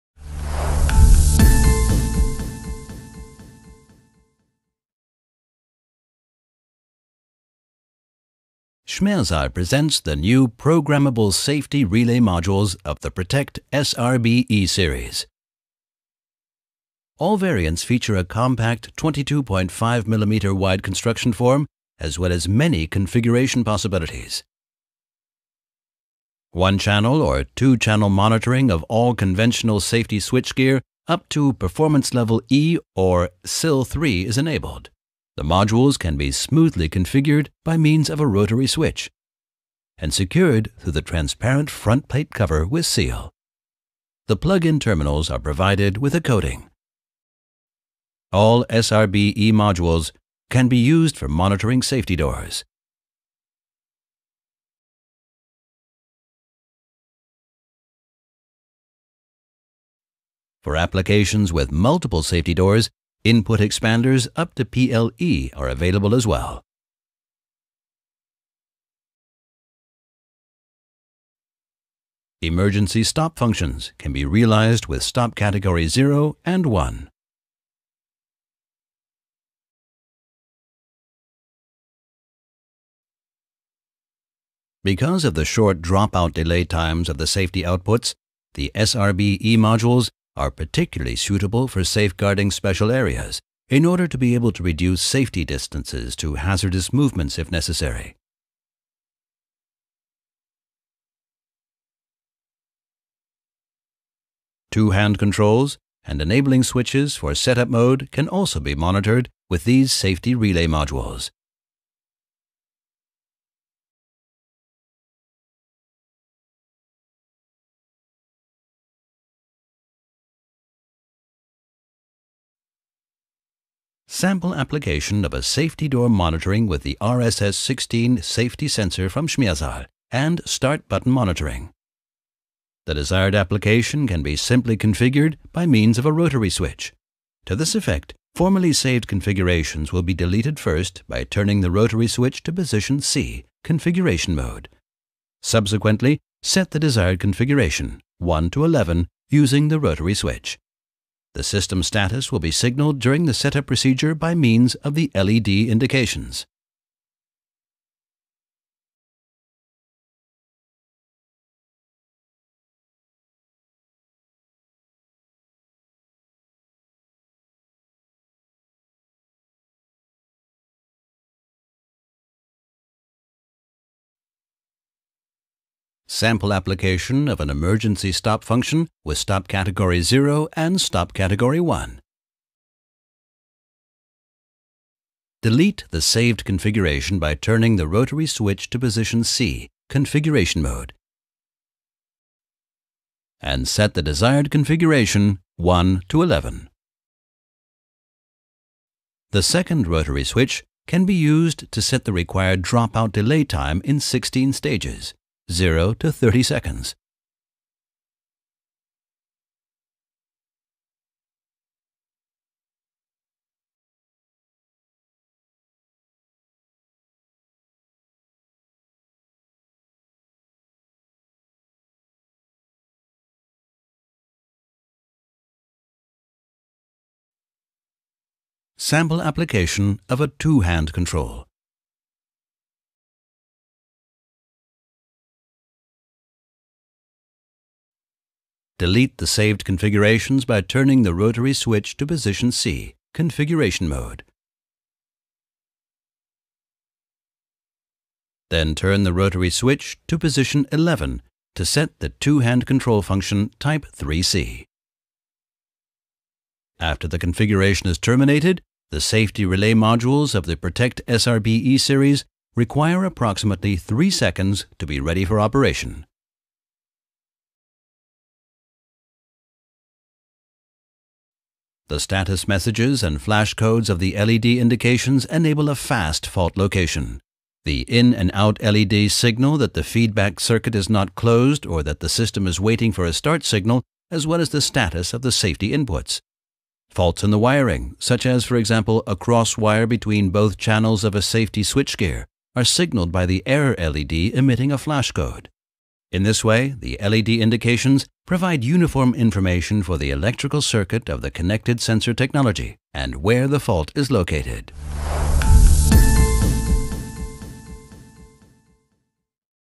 Schmersal Product Animations PROTECT SRB E
Safety Relay สามารถใช้ร่วมกับอุปกรณ์เซฟตี้ได้ทุกแบบ
Safety switch,Safety sensor,Safety light curtains, Emergency stop, Two hand switch